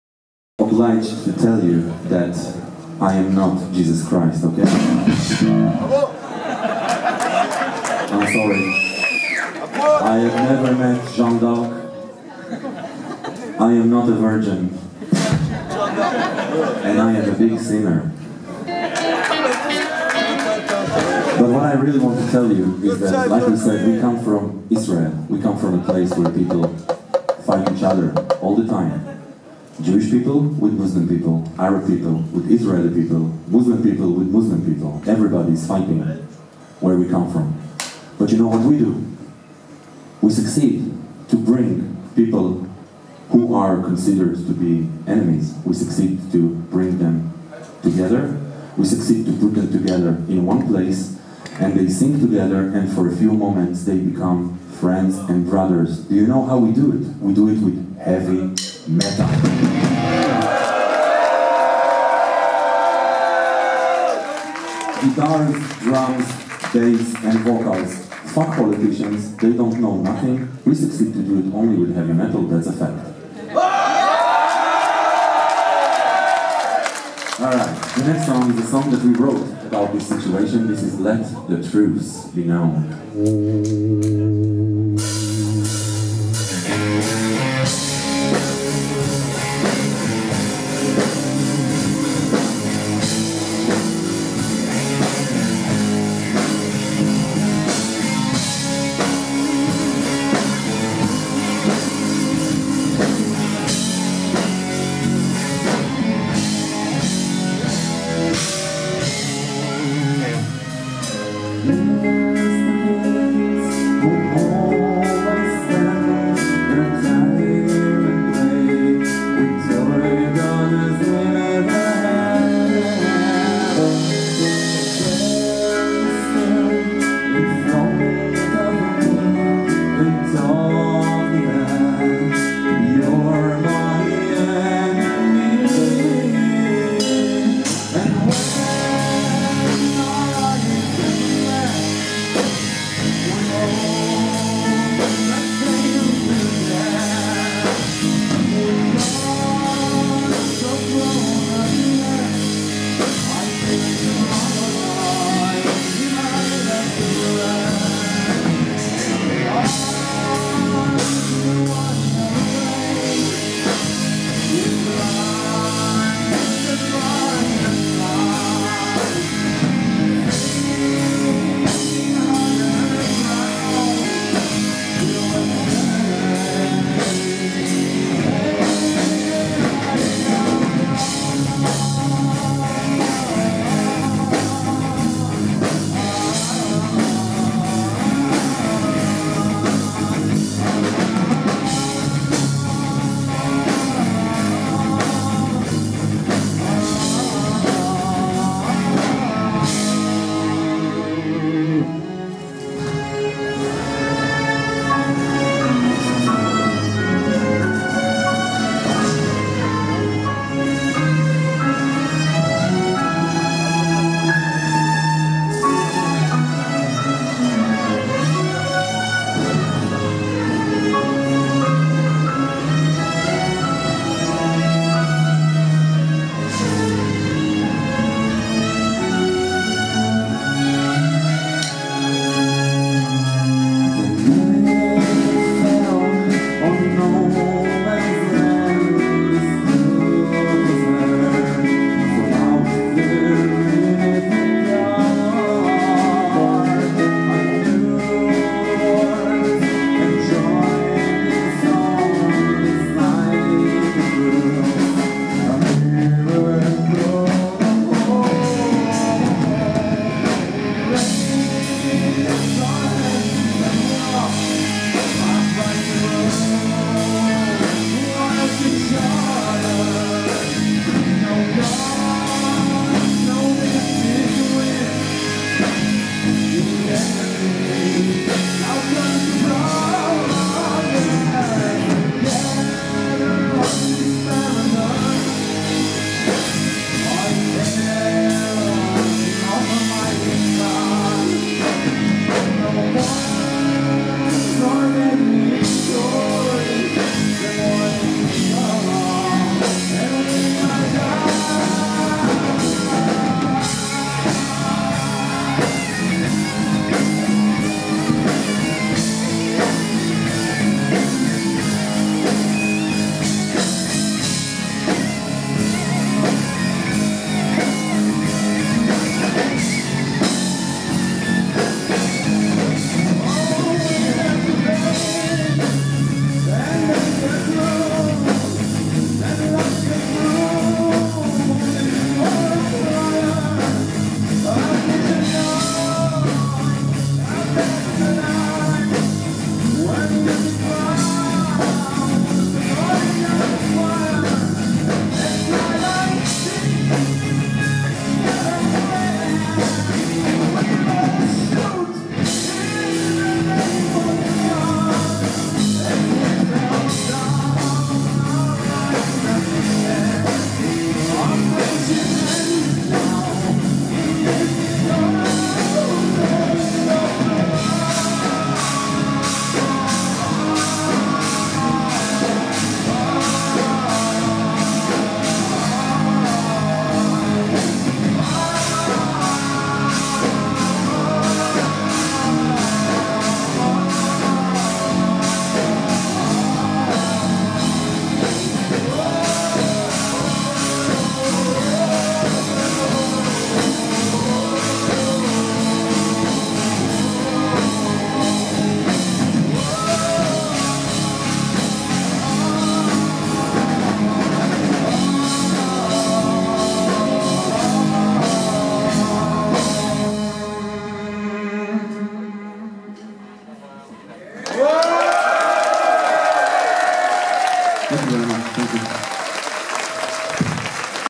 Orphaned Land live le 05/09/15 au Festival MFEST à Rouziers-De-Touraine
(Oriental Metal d' Israël)
Organisé par Mfest Asso !!!